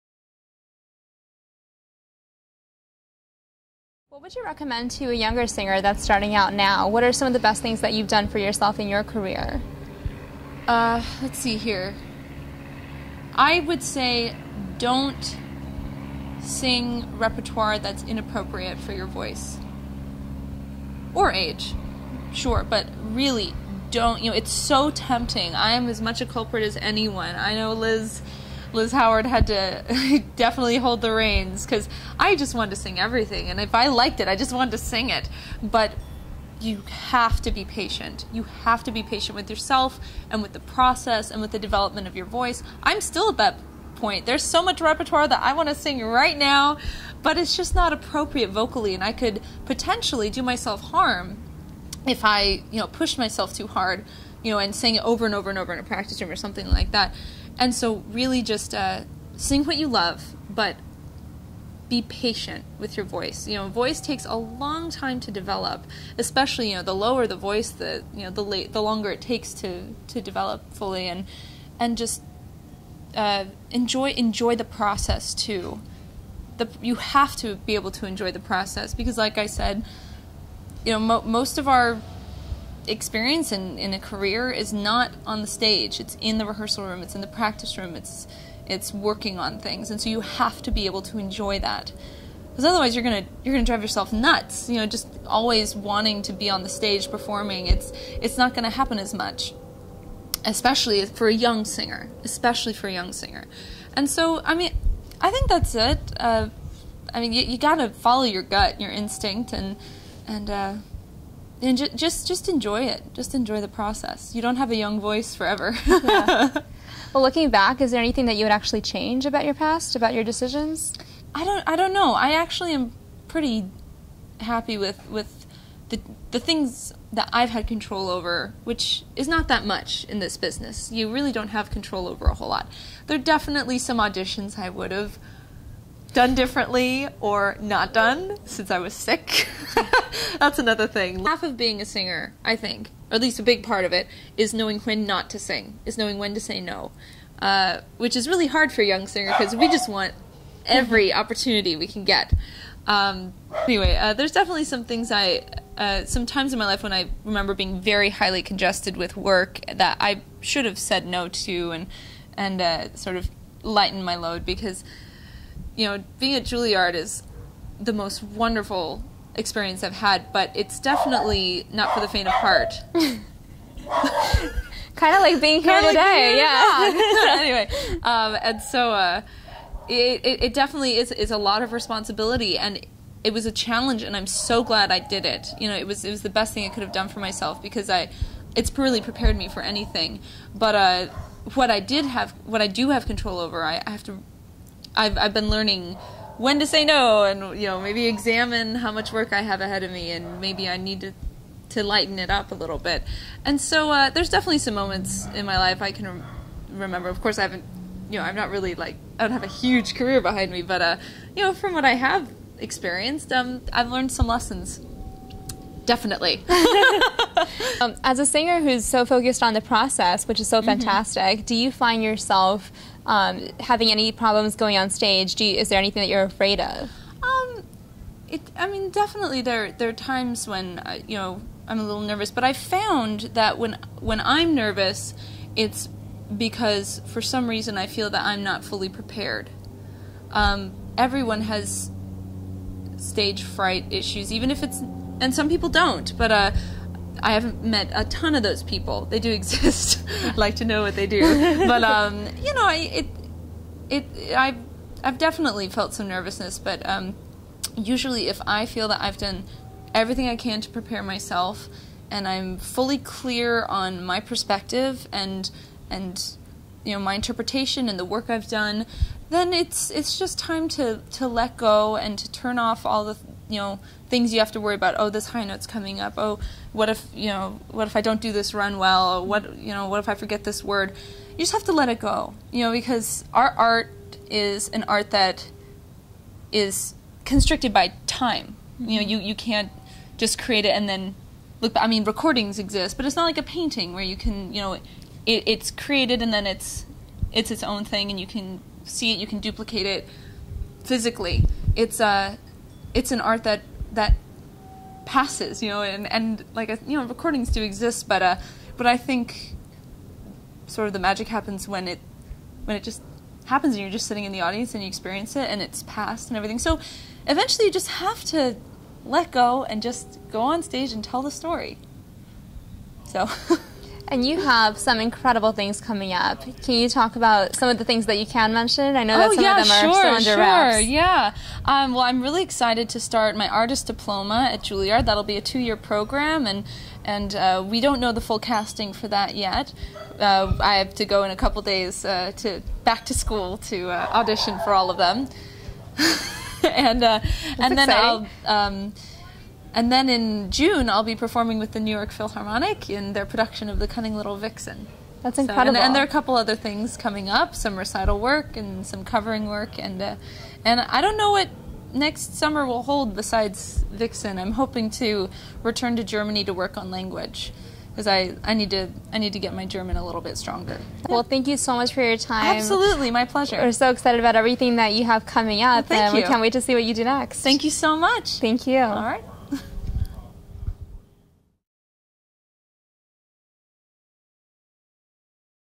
iCadenza interview with Devon Guthrie Part 2
Julia interviews soprano Devon Guthrie, whose training includes a Bachelor of Music degree from the Manhattan School of Music and a Master of Music degree from Juilliard. A student of Marlena Malas, Devon has recently participated in the Santa Fe Opera's Singer Apprentice program and prior to that performed at Tanglewood under the baton of James Levine. In this interview, she discusses her approach to new repertoire, her fears on stage, her advice to younger singers, her love for the rehearsal room, and the importance of art song repertoire.